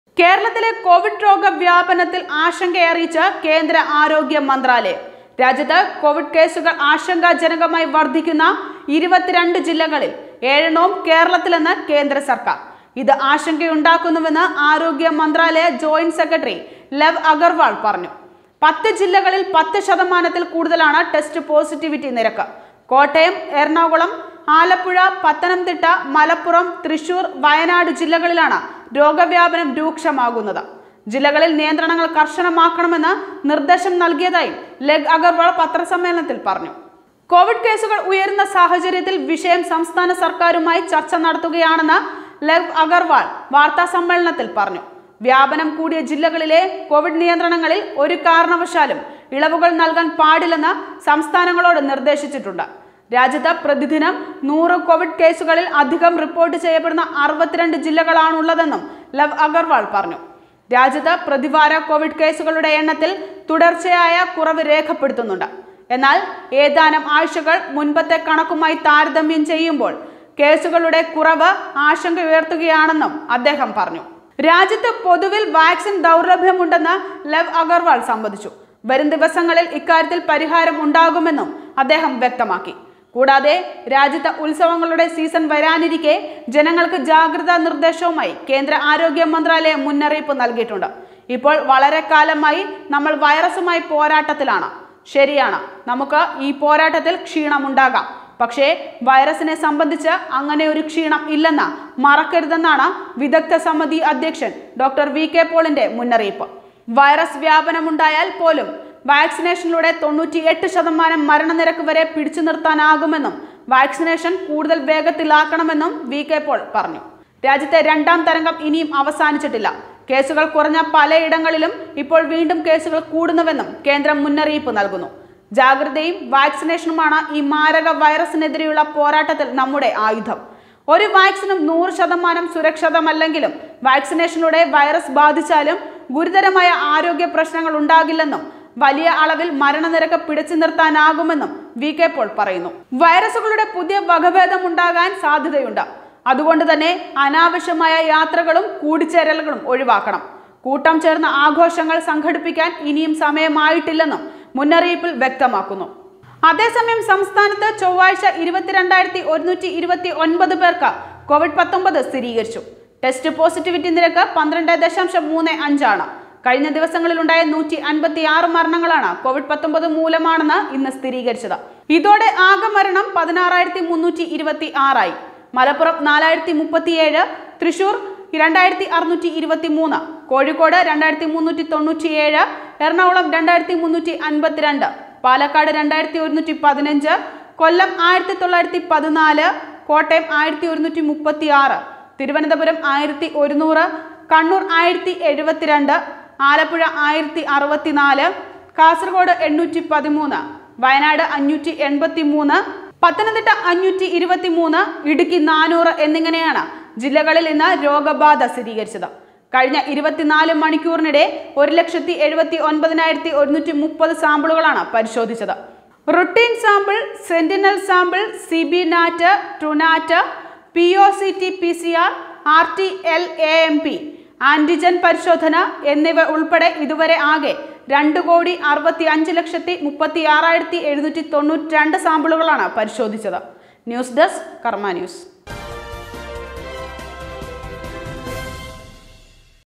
കേരളത്തിൽ വൈറസ് പിടിമുറുക്കുന്നു; ആശങ്കയറിയിച്ച്‌ കേന്ദ്രം
സംസ്ഥാനത്ത് വീണ്ടും രോഗബാധിതരുടെ എണ്ണം കൂടുന്നു; കേസുകള്‍ ആശങ്കാജനകമായി വര്‍ധിക്കുന്നത് 22 ജില്ലകളിൽ; ഏഴും കേരളത്തില്‍, ആശങ്കയറിയിച്ച്‌ കേന്ദ്രം